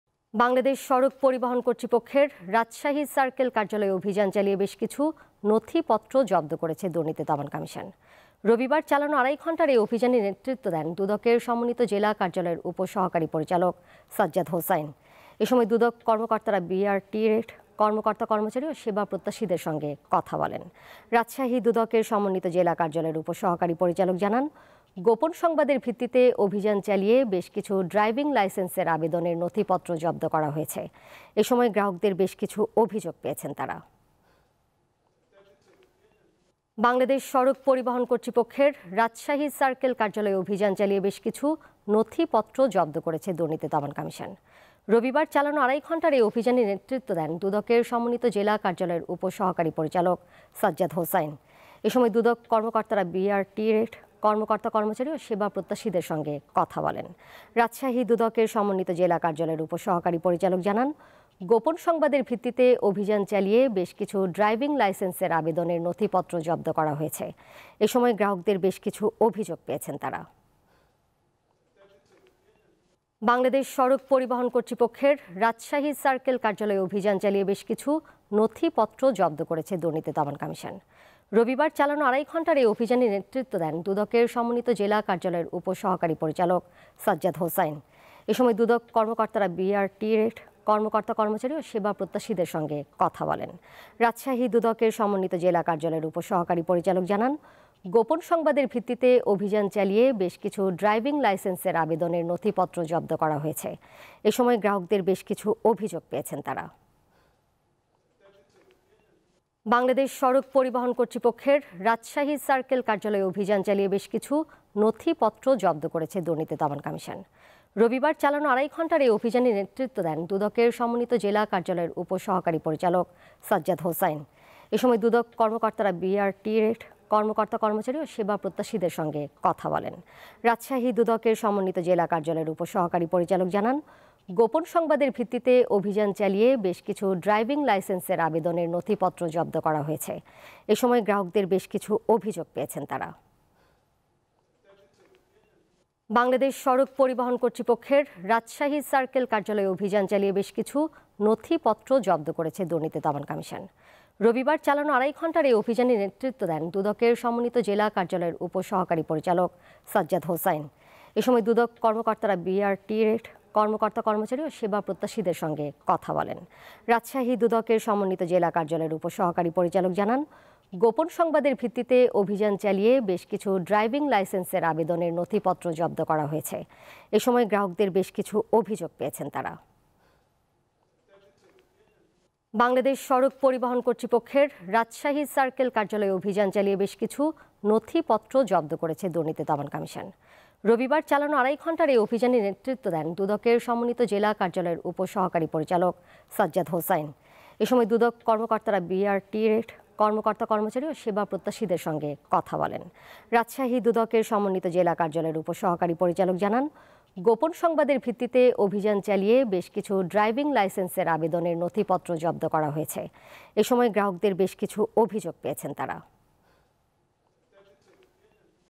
বিআরটিএ'র রাজশাহী সার্কেল কার্যালয়ে দুদকের অভিযান, নথিপত্র জব্দ | Rajshahi BRTA | Deepto News
Address:
Deepto TV
7/A/GA Tejgaon Industrial Area,